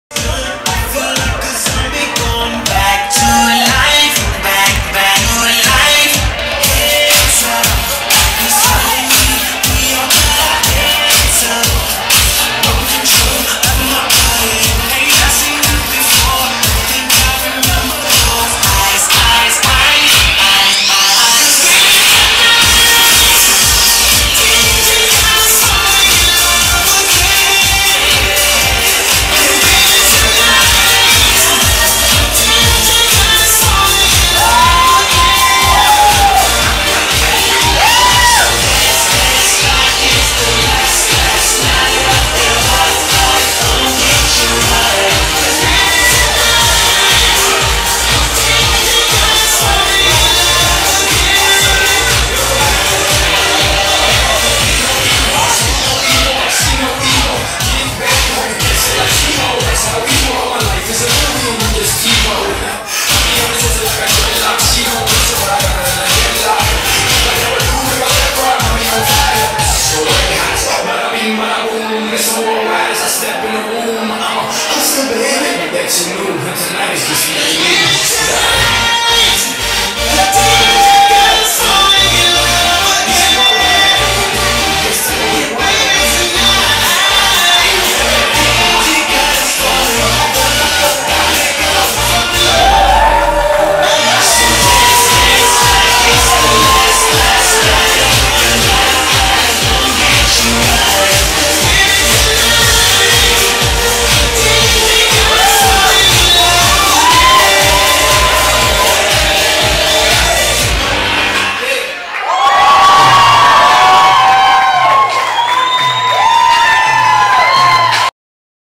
Keep it Sexy Party @ La Casa de Tula in Miami
What an event at La Casa de Tula in Miami- "KEEP it SEXY" TULA Party--
3 Bands played, Free Sushi, Complementary Masseuse, Pole Dancing, a Burlesque Set, Free Chocolate Covered Strawberries, Free Prize Give-Aways, and a HELL of a GOOD TIME! Now u know what all the fuss was about! Thanks Miami!